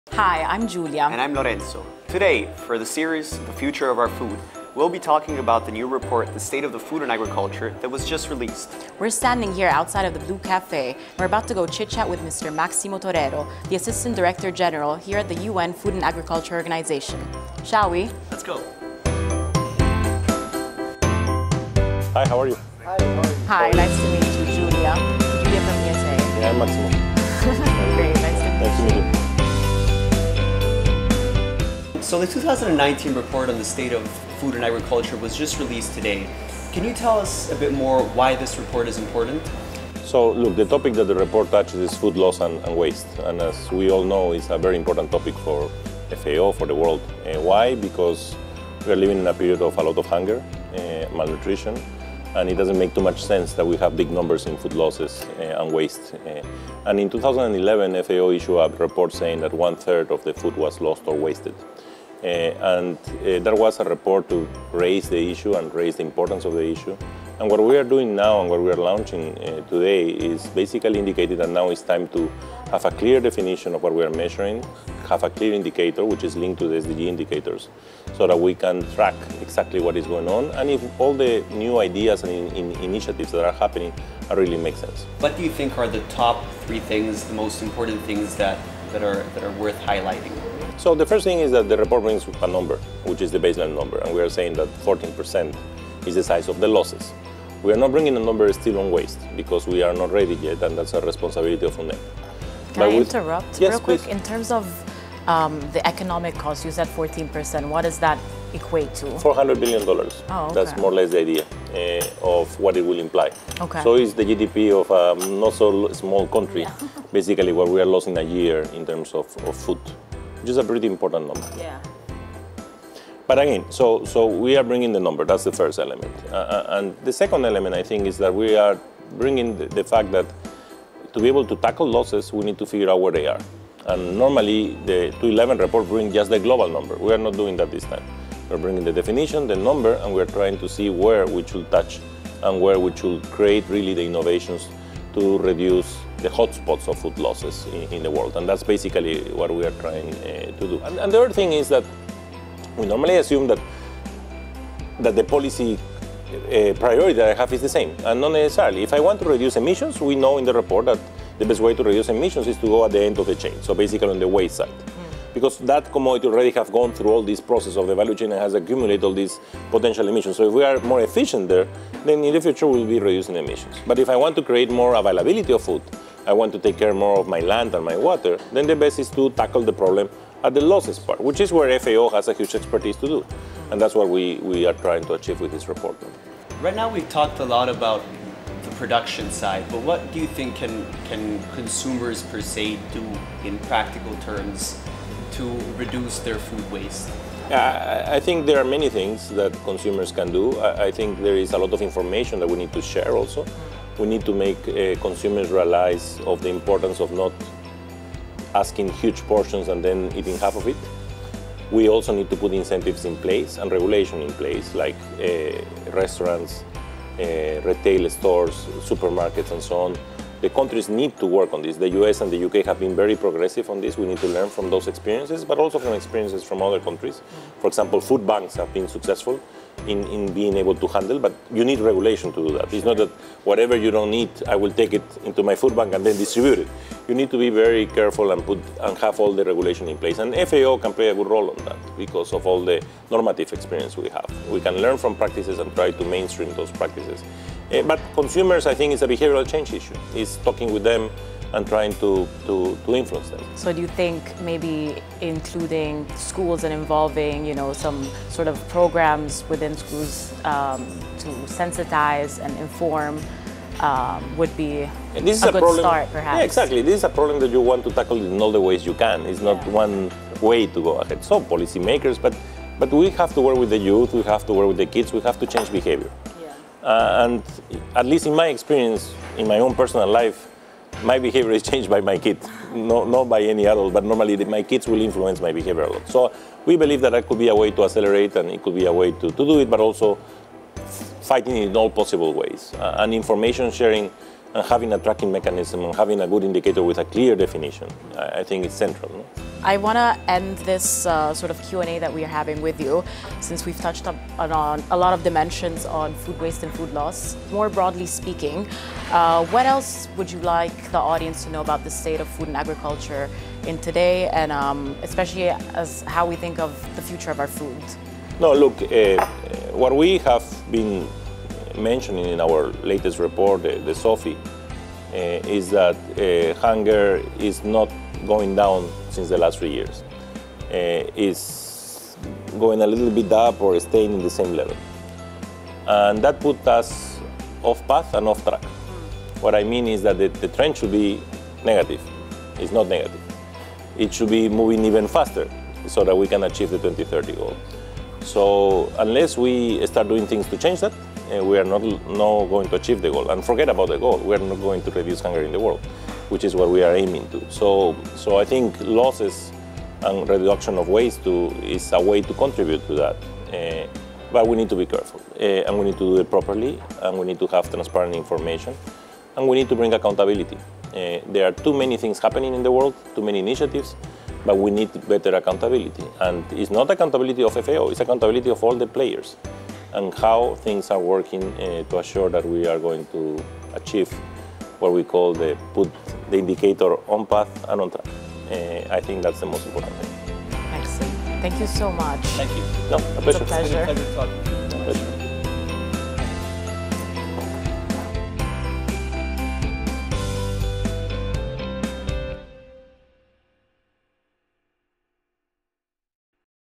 FAO Youth Series - How can we measure food loss & waste? Interview with Máximo Torero
In this episode of the Future of Our Food, Giulia and Lorenzo have a coffee with Dr. Máximo Torero, Assistant Director General at the Food and Agriculture Organization of the UN. They talk about the financial cost of food loss (hint: it’s in the billions), the new 2019 State of Food and Agriculture Report (SOFA 2019), and recent trends in world hunger.

How can we change our behavior? And who holds the power to combat hunger and climate change? Watch the video to find out.